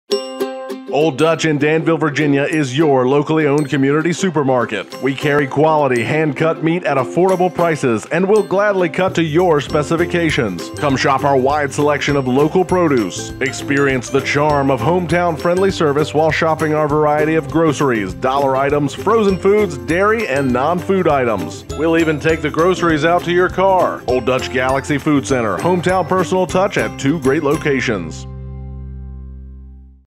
Old Dutch Supermarkets - Danville, VA
Old Dutch Supermarkets have served the great citizens of Danville, Pittsylvania County and surrounding areas since for 1958. Old Dutch #3 (North Main St.) and Old Dutch #4 (West Main St.) are locally owned and operated grocery stores with fresh produce and fresh meat cut in house daily. Our full service meat department tha will give you excellent customer service and will be happy to help you with any custom cuts of meat you would like.